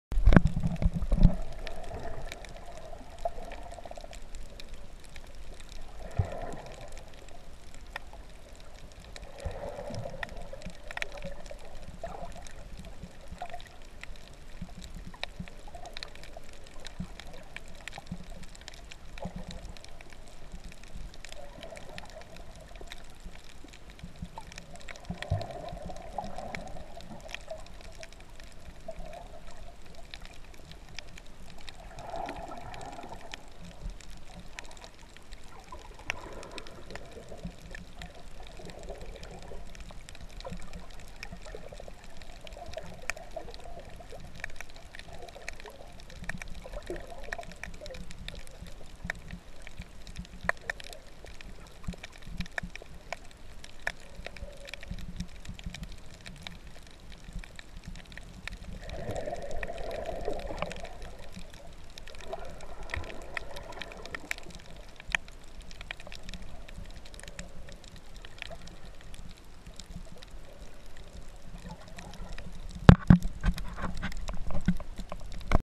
Eel At Kealakekua Bay, Hawaii
Following an eel at Kealakekua Bay, Hawaii.